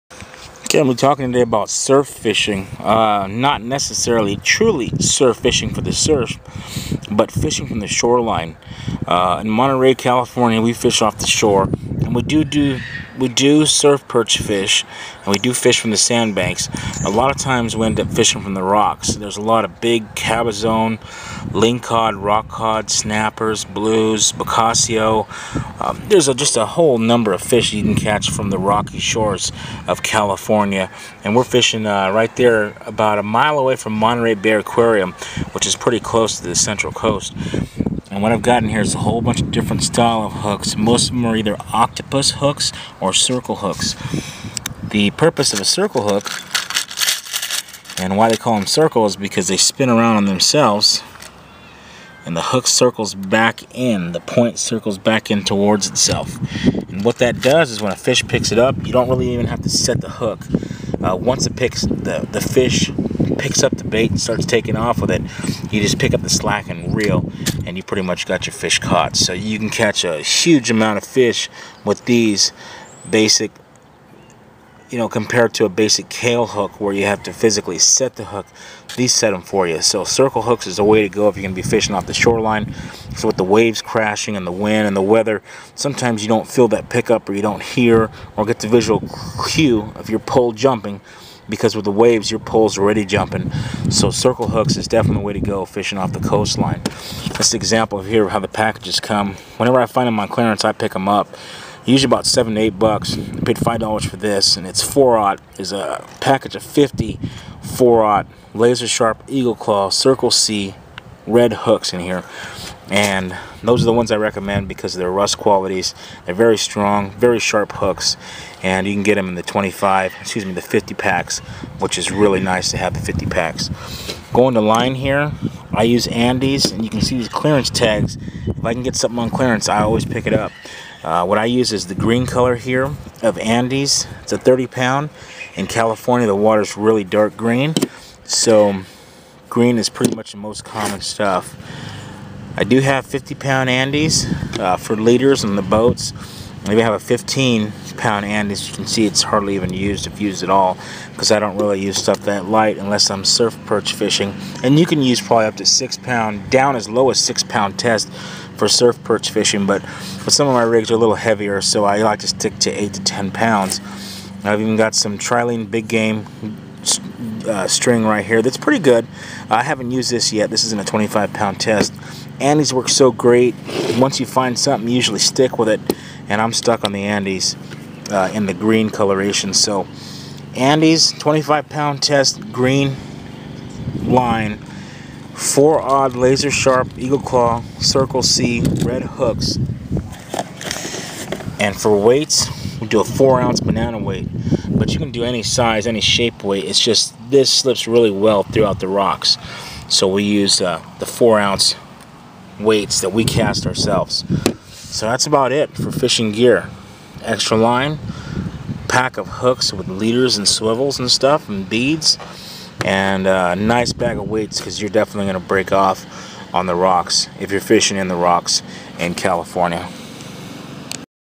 OCEAN FISHING GEAR!
This is what I use in California. The fishing is awesome here and we limit out often. Ling Cod season opened April 1, 2011 in our fishing area. I am just showing you what we use and what I recommend for ling cod and other RCG complex fish.